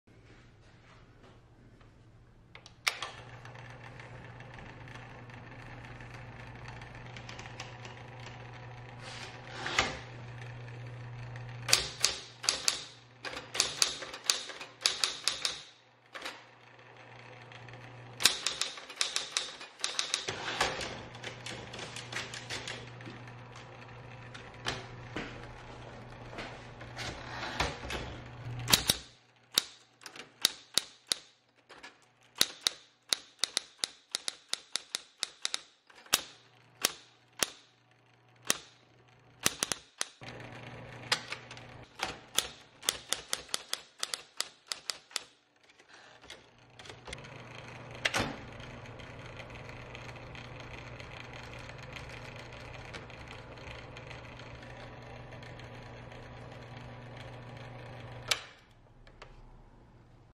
Vintage Electric Typewriter Smith Corona Coronet Automatic 12 For Sale
eBay listing

All Keys work fine, scratched up and faded in a few corners but the machine works perfectly.